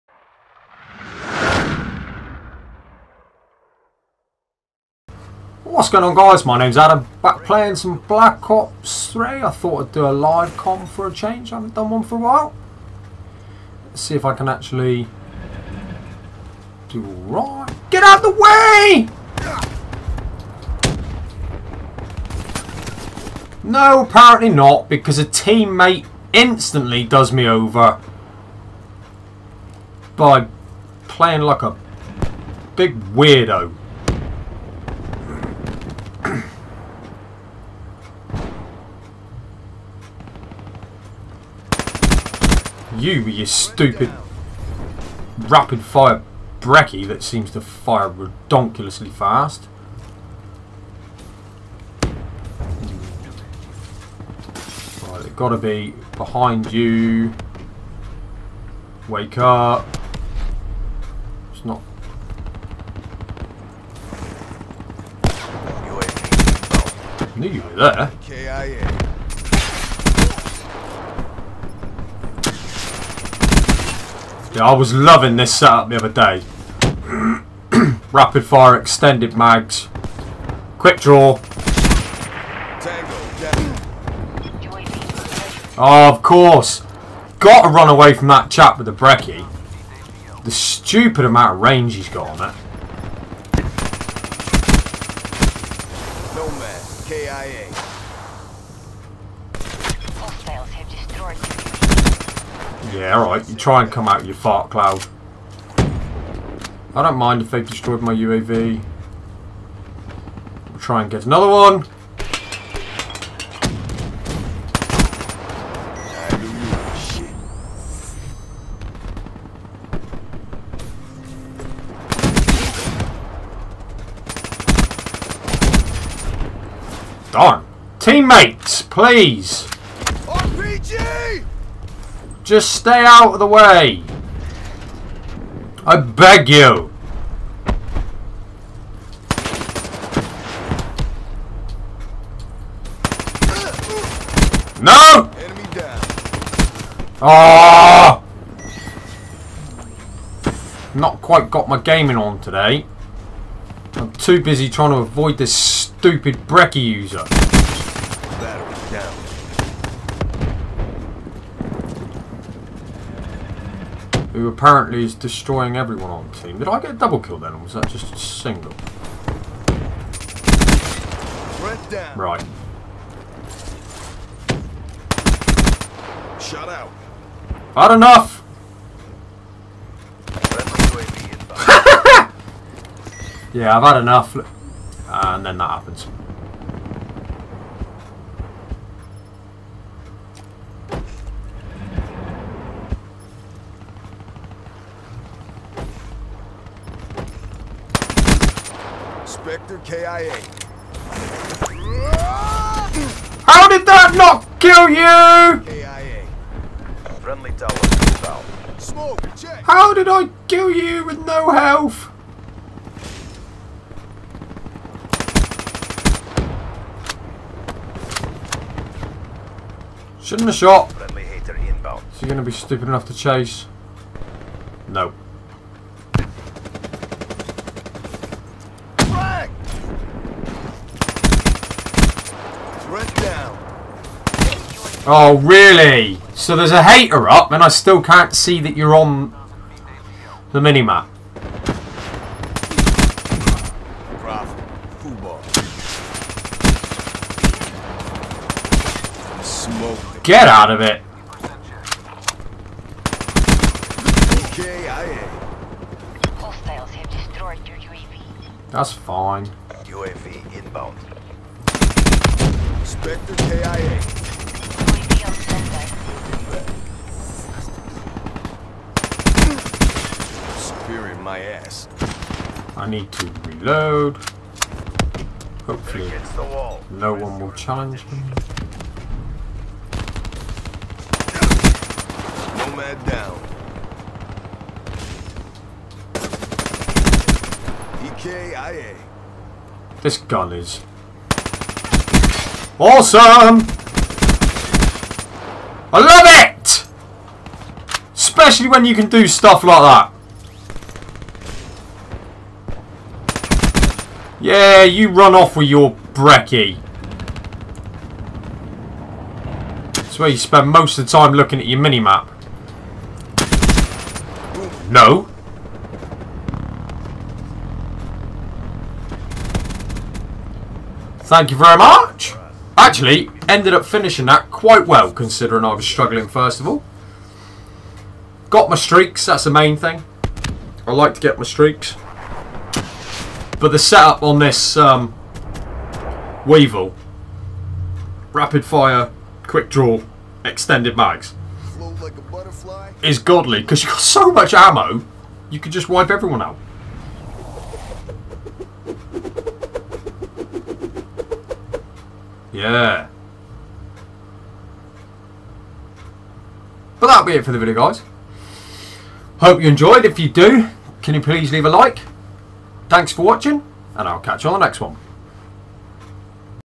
Combine with the weevil
Weevil is an awesome gun use it with quick draw, extended mags and rapid fire ENJOY